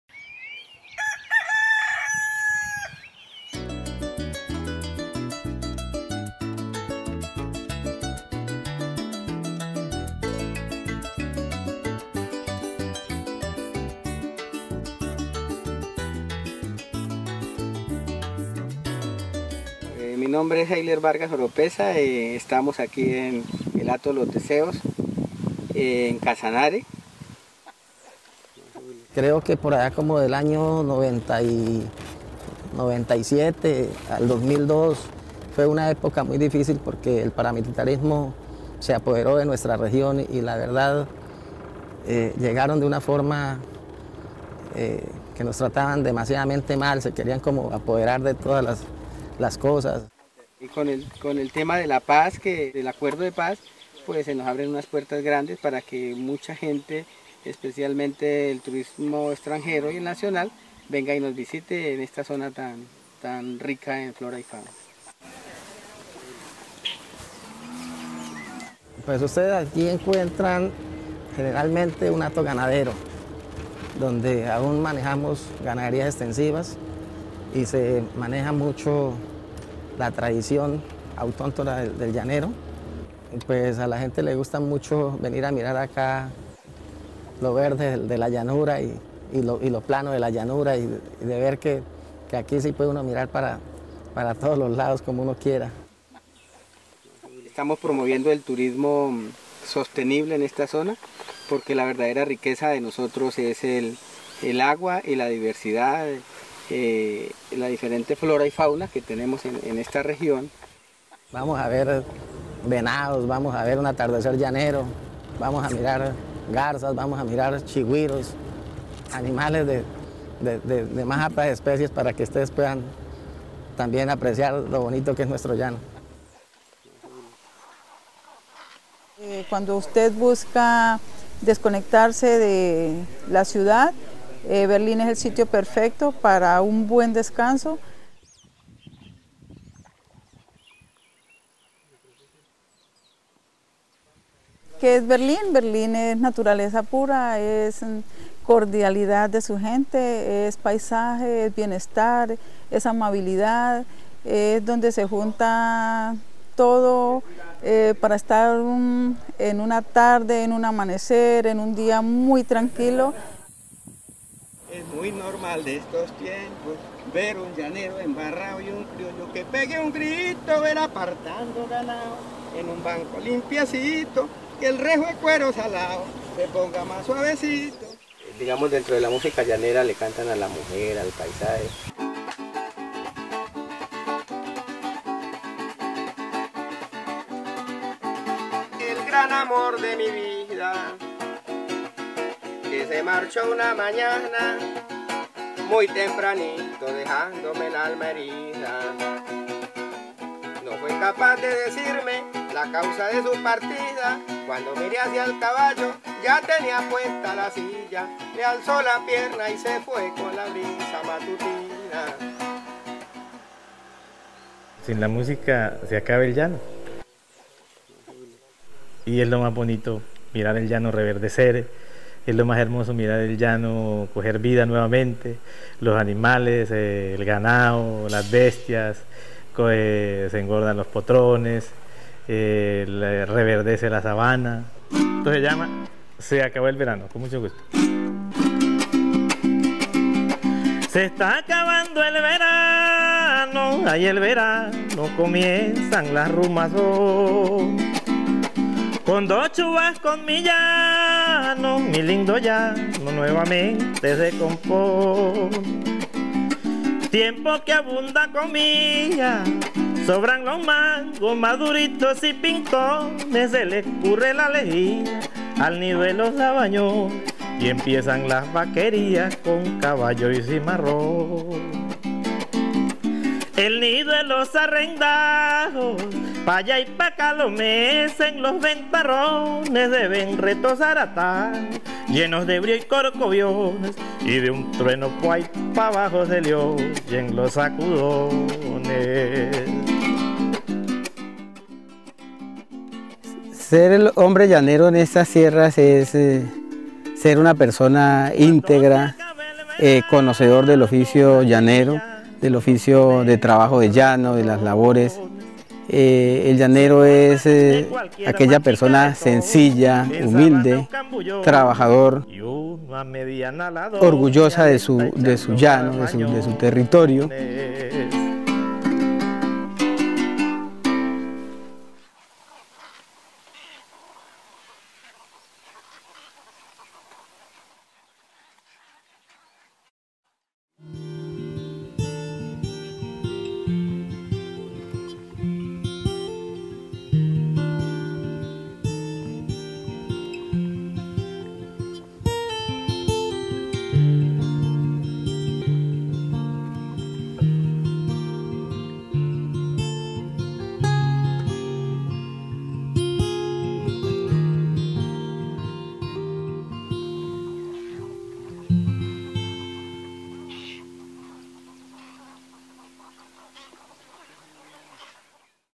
Casanare, un safari llanero por la sabana
Conozca las historias que ilustran cómo el turismo se ha vuelto una nueva opción de vida en Casanare, Meta y Montes de María en el especial multimedia ‘Viajes para renacer’: